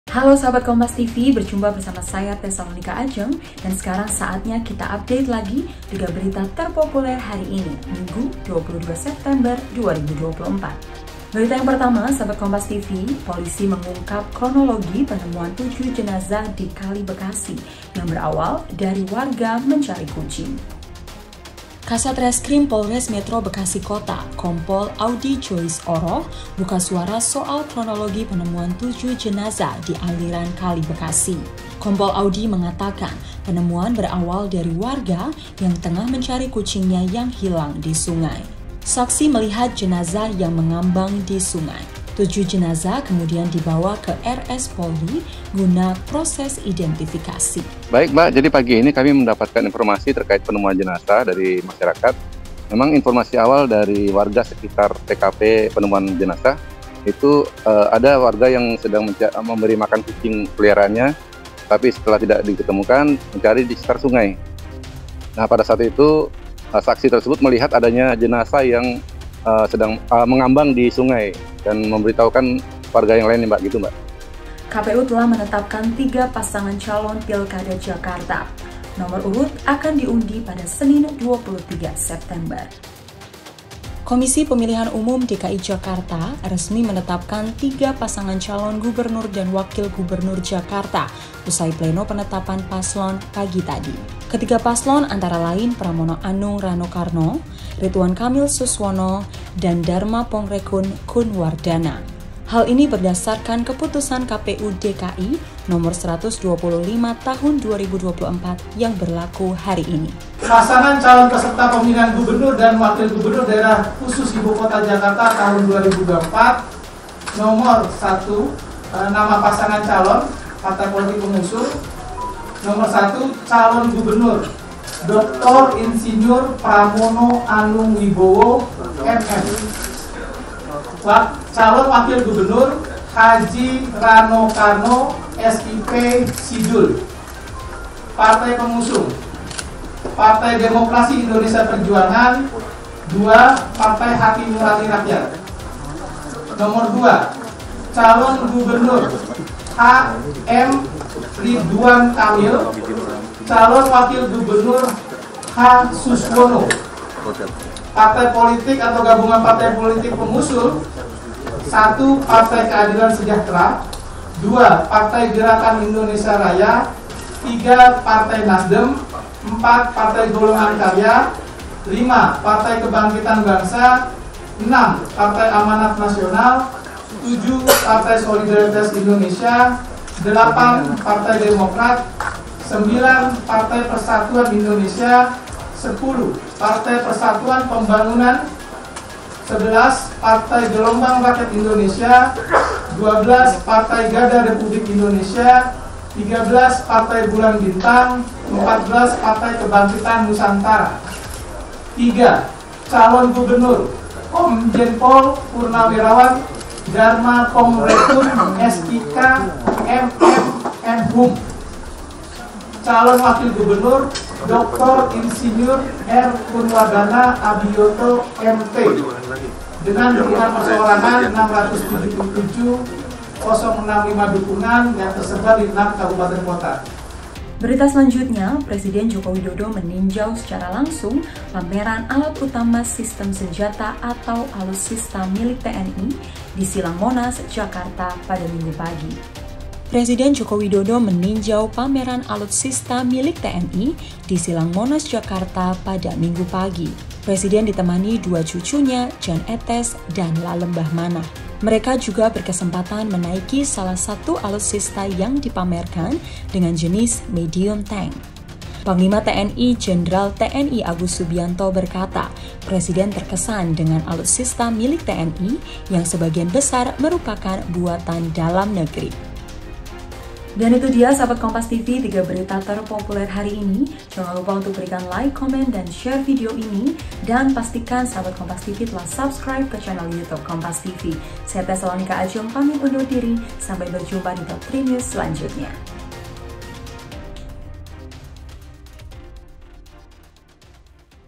Kronologi 7 Jenazah di Bekasi, KPU Jakarta Tetapkan 3 Pasangan, Jokowi Tinjau Pameran Alutsista
JAKARTA, KOMPASTV - Berikut tiga berita terpopuler yang terjadi pada Minggu (22/9/2024);

Berita pertama, Kasat Reskrim Polres Metro Bekasi Kota, Kompol Audy Joize Oroh mengatakan penemuan 7 jenazah di aliran Kali Bekasi, berawal dari warga yang tengah mencari kucingnya yang hilang di sungai.

Berita kedua, Komisi Pemilihan Umum DKI Jakarta resmi menetapkan tiga pasangan calon gubernur dan wakil gubernur Jakarta, usai pleno penetapan paslon, Minggu (22/9/2024). Ketiga paslon di antaranya Pramono Anung-Rano Karno, Ridwan Kamil-Suswono dan Dharma Pongrekun-Kun Wardana.

Berita ketiga, Presiden Joko Widodo meninjau pameran alutsista milik TNI di silang Monas Jakarta. Presiden ditemani dua cucunya Jan Ethes dan La Lembah Manah. Panglima TNI Jenderal Agus Subiyanto sampaikan Presiden terkesan dengan alutsista yang dipamerkan, karena mayoritas buatan dalam negeri.

Baca Juga KPU Jabar Tetapkan 4 Paslon Maju di Pilkada, Nomor Urut Diambil 23 September di